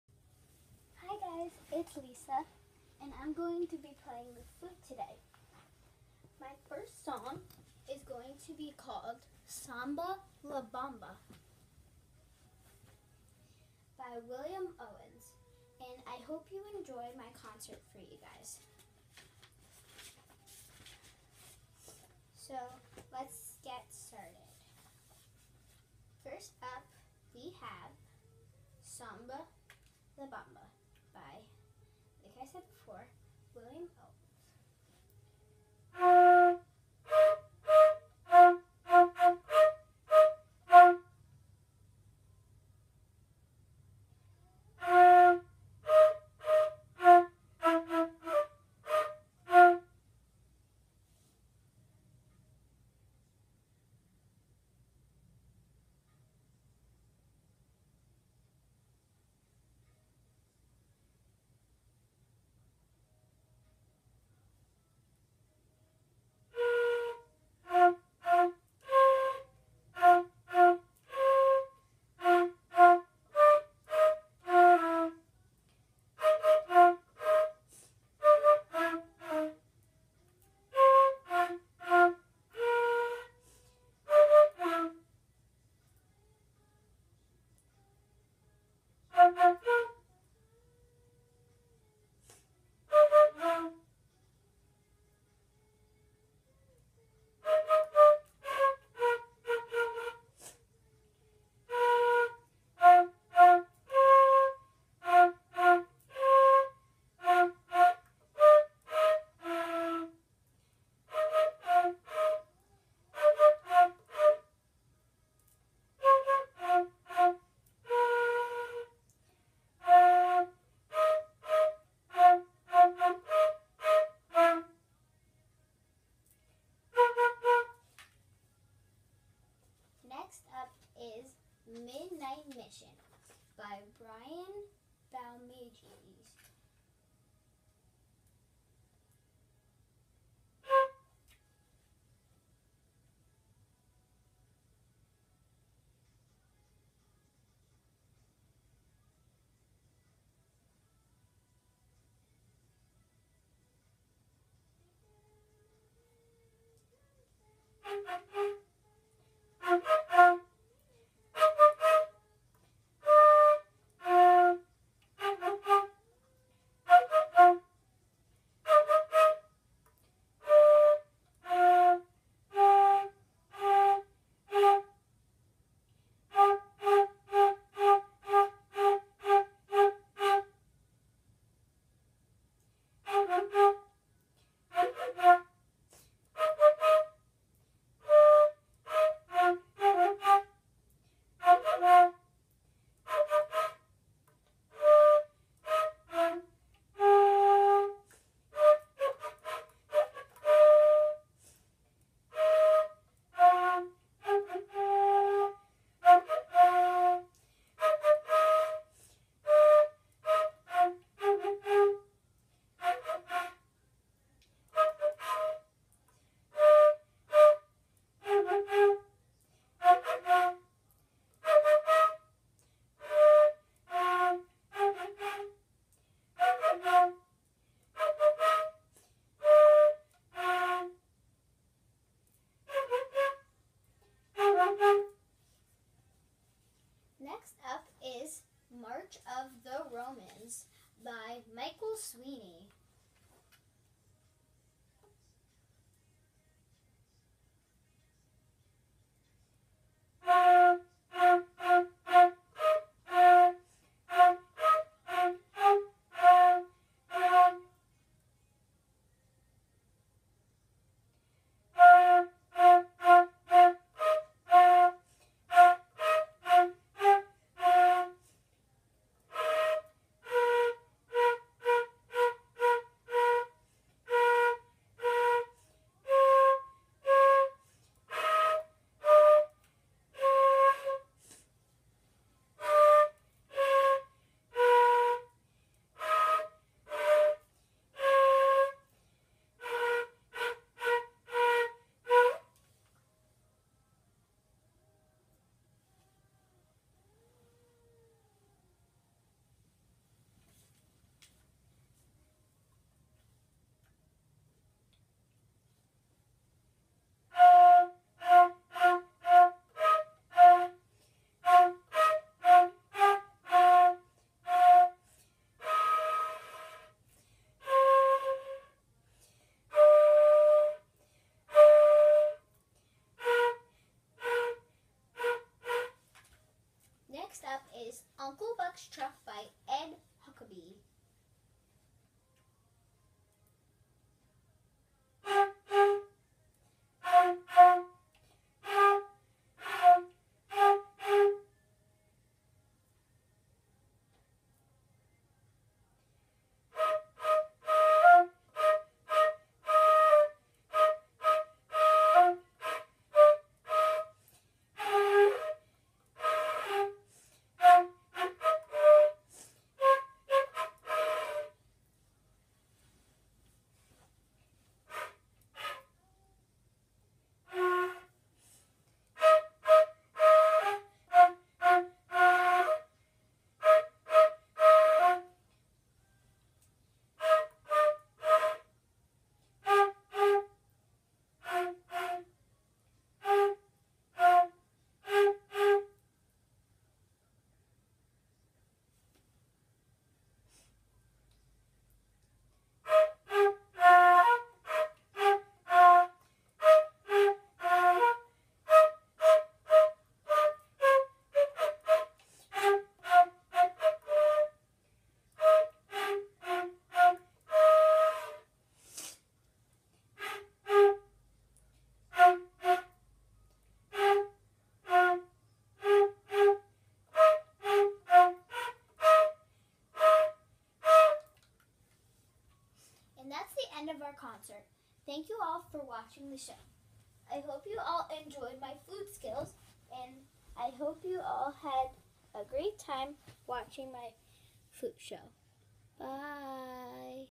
Flute Show
That's me playing my band music! Hope you like it!